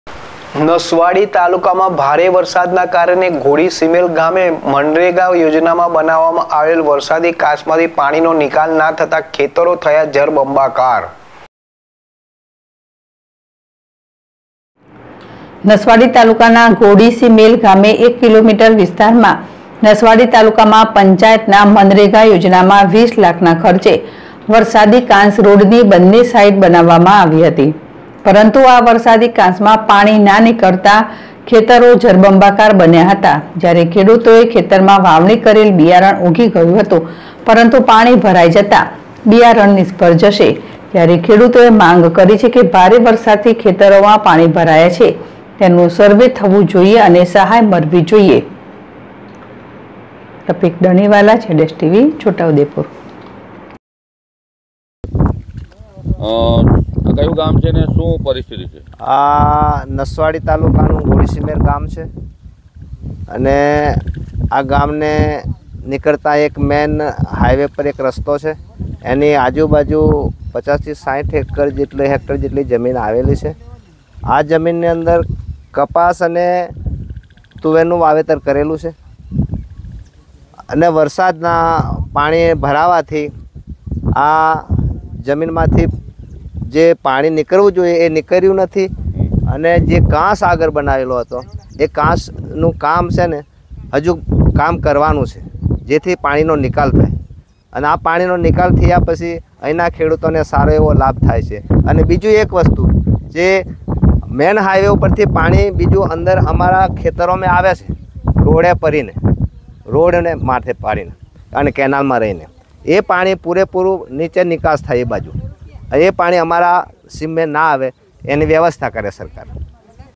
ખેડૂતોનો પાક નિષ્ફળ:ઘોડીસિમેલમાં વરસાદી કાંસ પાછળ કરાયેલો ખર્ચ પાણીમાં, 200 એકર ખેતર જળબંબાકાર
નસવાડી તાલુકા માં ભારે ખેડૂતોનો પાક નિષ્ફળ:ઘોડીસિમેલમાં વરસાદી કાંસ પાછળ કરાયેલો ખર્ચ પાણીમાં, 200 એકર ખેતર જળબંબાકાર